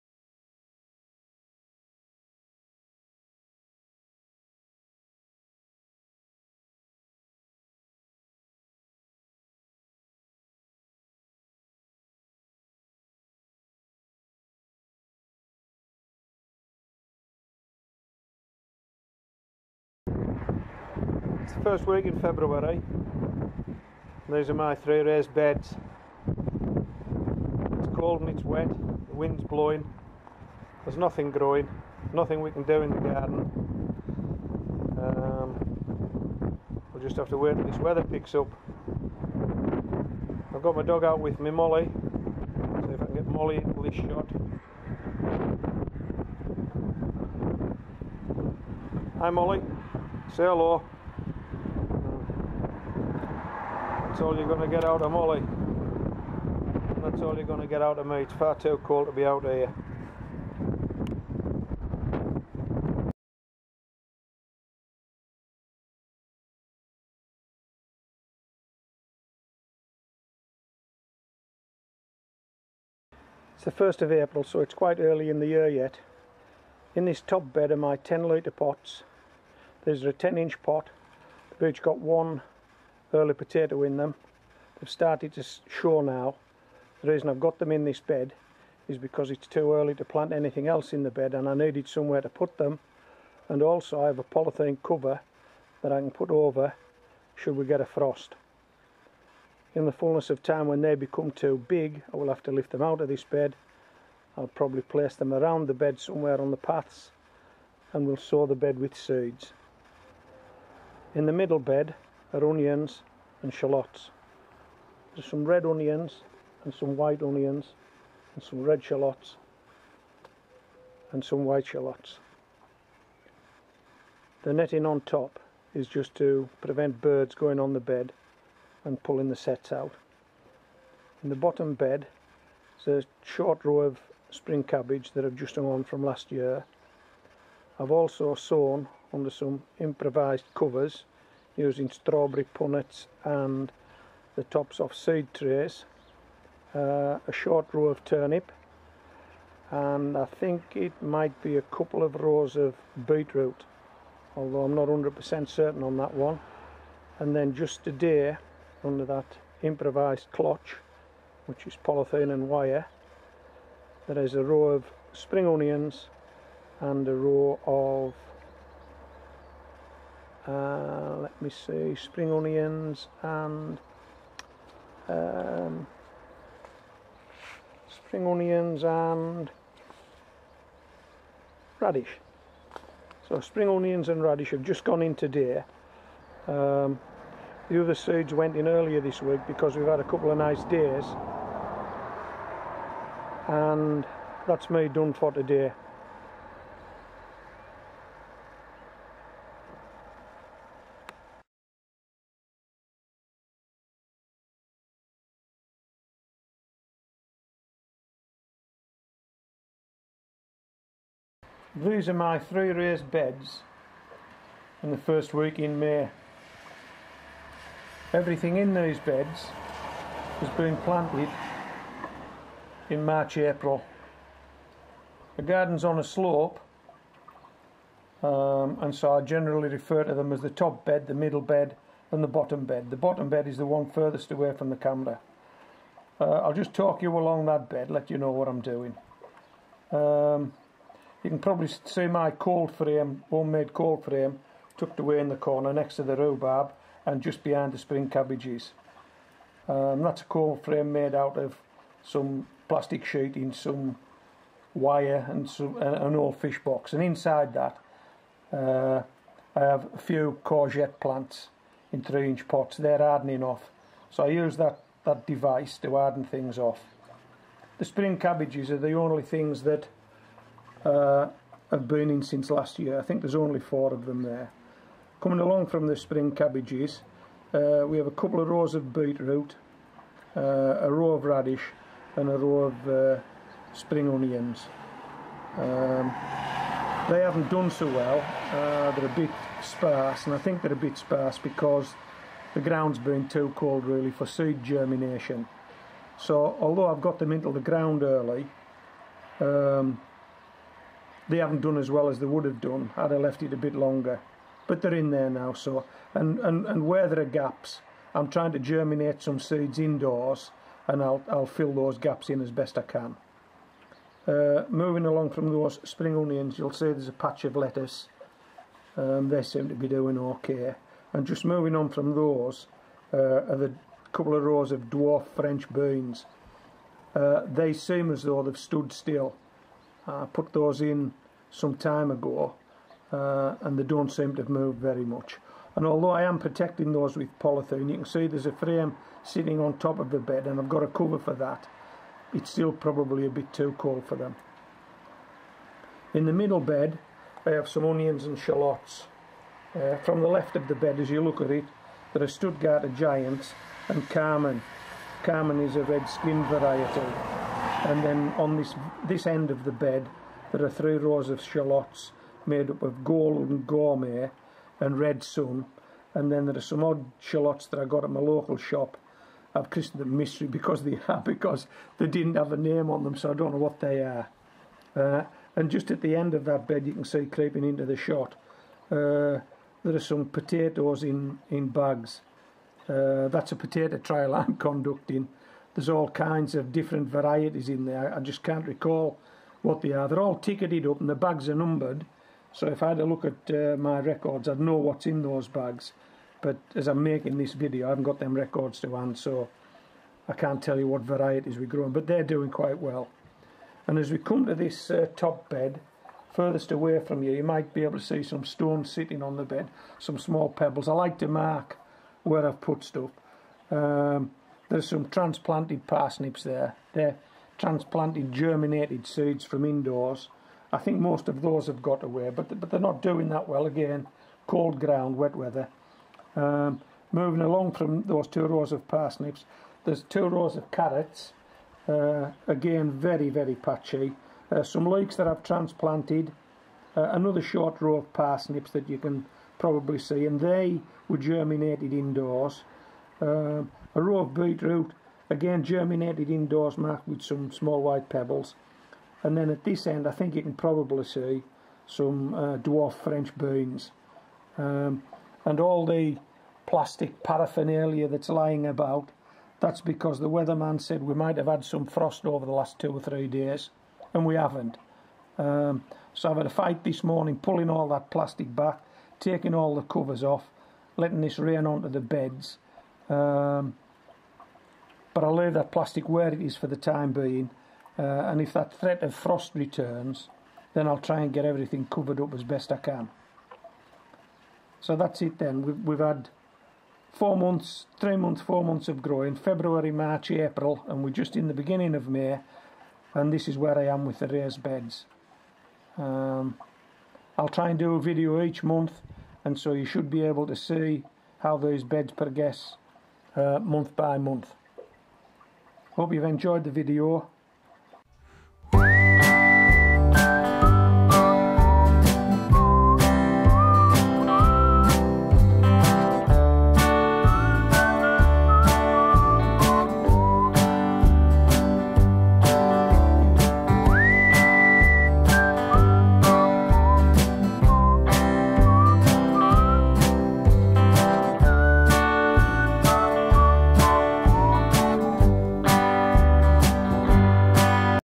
Spring Raised bed update. Parsnips, carrots, leeks, beetroot, onions, and more.
It's a bit early in the year with cold soil and cold winds but starting seeds of indoors has enabled me to fill my raised beds with short rows of parsnips, carrots, leeks, beetroot, dwarf broad beans, dwarf runner beans, onions, shallots, spring onions, radish and lettuce. Fingers crossed!  If you want to see next month's update click the